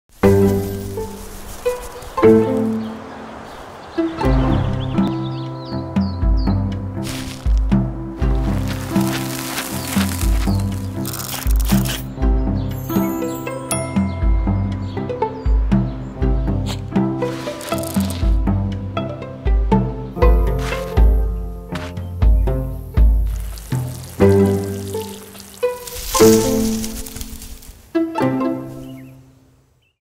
Miraculous Gardening - How to Plant Broccoli
Planting broccoli in your garden will reward you with fresh flavor you won’t find at the store. Discover the best way to plant broccoli, including spacing and planting depth.